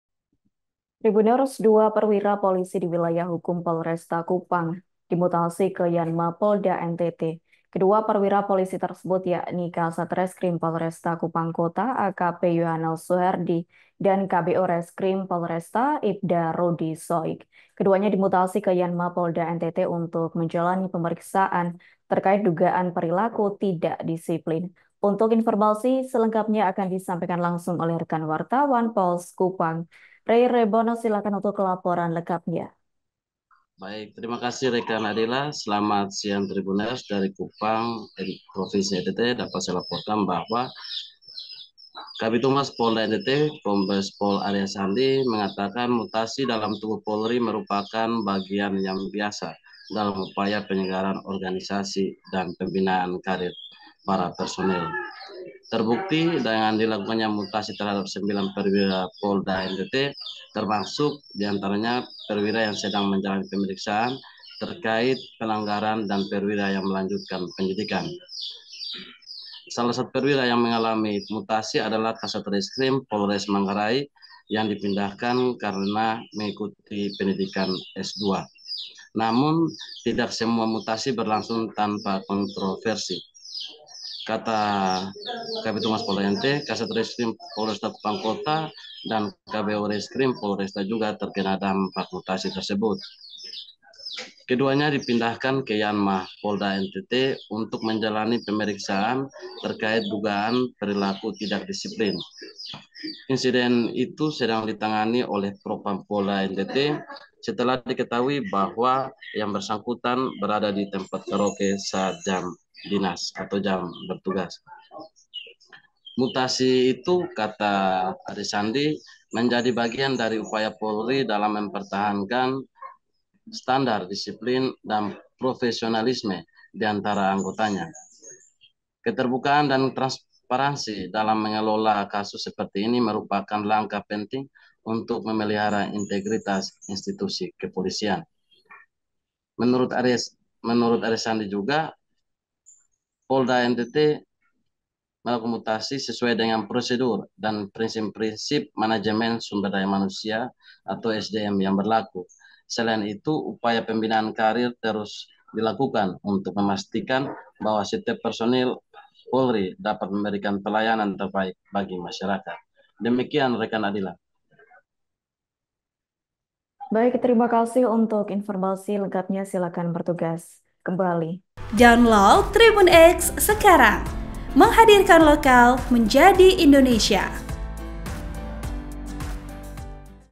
Diduga Tak Disiplin, 2 Perwira Polresta Kupang Kota Dimutasi ke Yanma Polda NTT: Pembinaan Karier
Laporan wartawan Pos Kupang - Rey Rebon
TRIBUN-VIDEO.COM- Dua Perwira Polisi di wilayah hukum Polresta Kupang Kupang di mutasi ke Yanma Polda NTT.

Kedua perwira polisi tersebut ialah; Kasat Reskrim Polresta Kupang Kota, AKP Yohanes Suhardi dan KBO Reskrim Polresta, Ipda Rudy Soik.

Keduanya dimutasi ke Yanma Polda NTT untuk menjalani pemeriksaan terkait dugaan perilaku tidak disiplin.

Program: Live Update
Host: Adila Ulfa Muna Risna
Editor Video: Latif Ghufron Aula
Uploader: Restu Riyawan